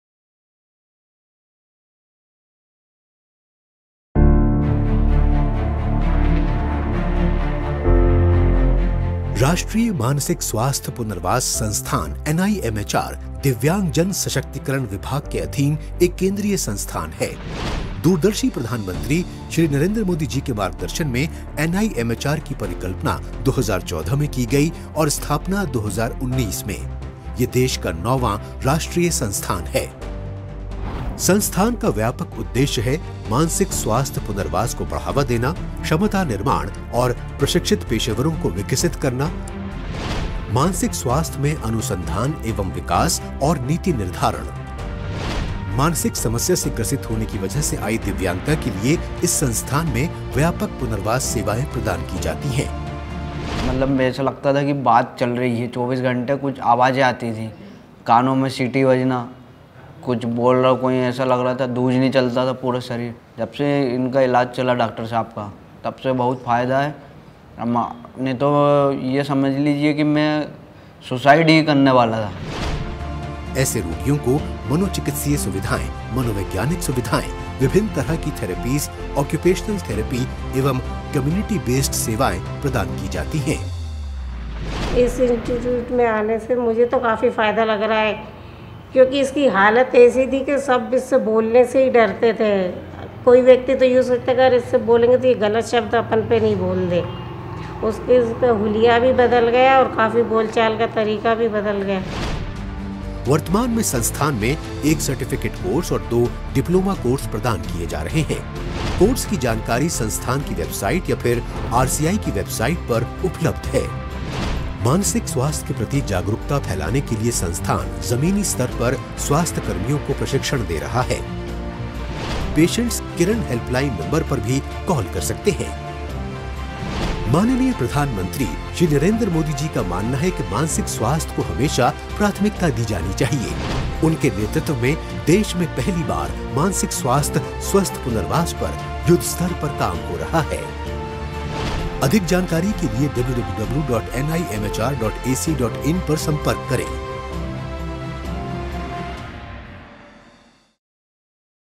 NIMHR Sehore- Empowering Minds: NIMHR Leading Mental Health Research and Rehabilitation in India
The National Institute for Mental Health and Rehabilitation (NIMHR) is an institute in India dedicated to mental health research and rehabilitation. It focuses on advancing knowledge and practices related to mental health and providing rehabilitation services to individuals with mental health issues.